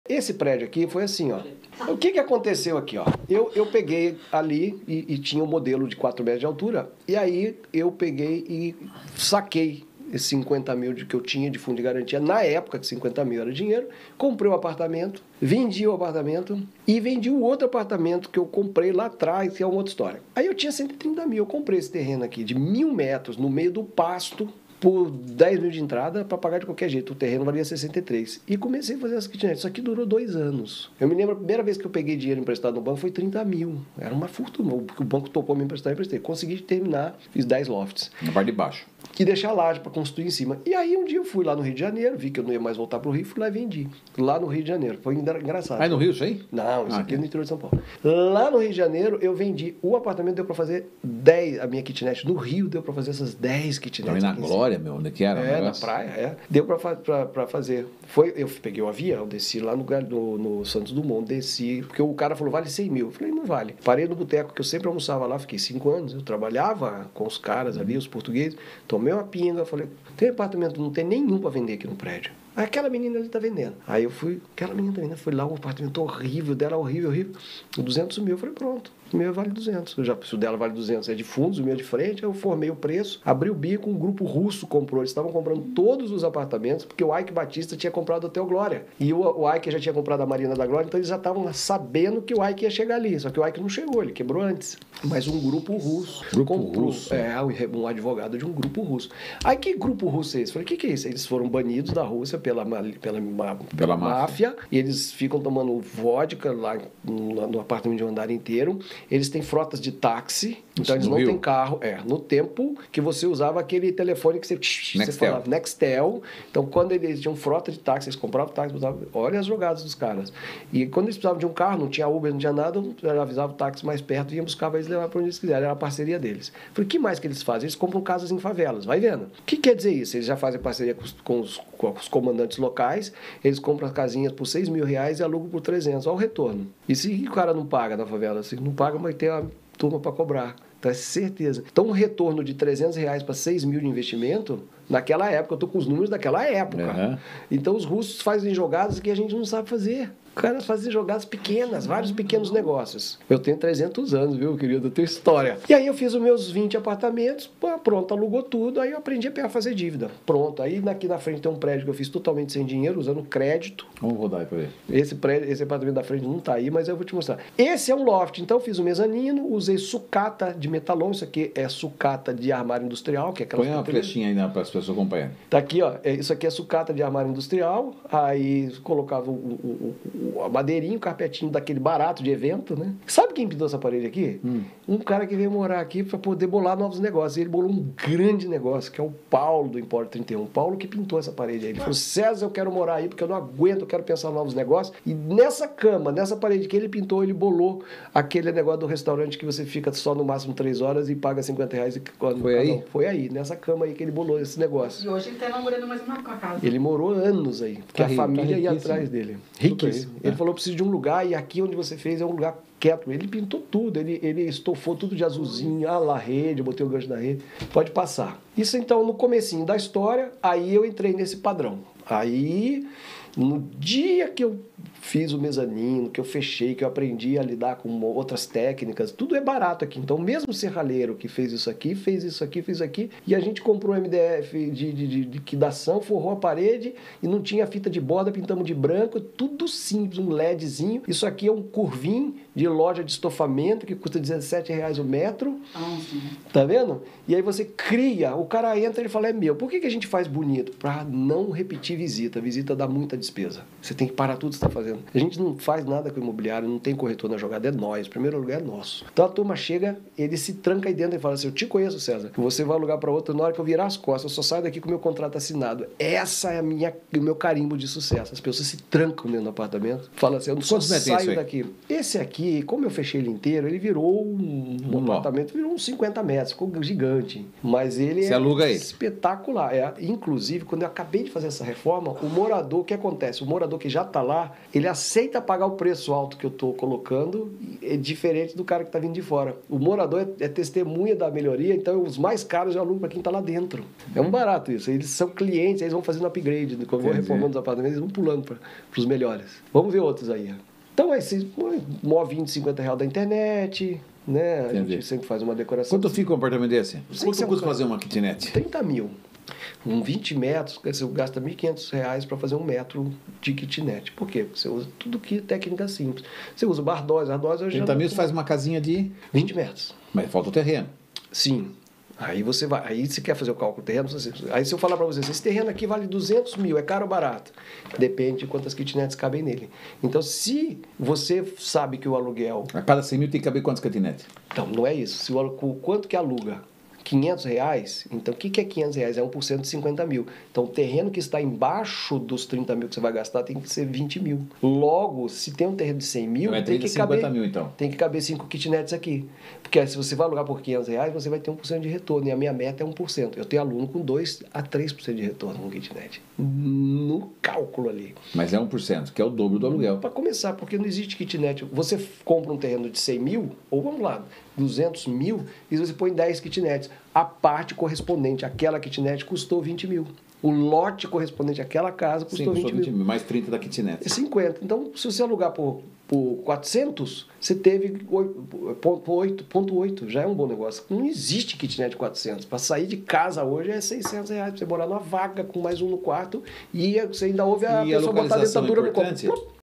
Como comecei a CONSTRUIR e ESTRUTURAR kitnets?
Conheça a minha história investindo em kitnets! Descubra como e porque comecei e me apaixonei por esse mercado!

Responsável: Cesar Barroso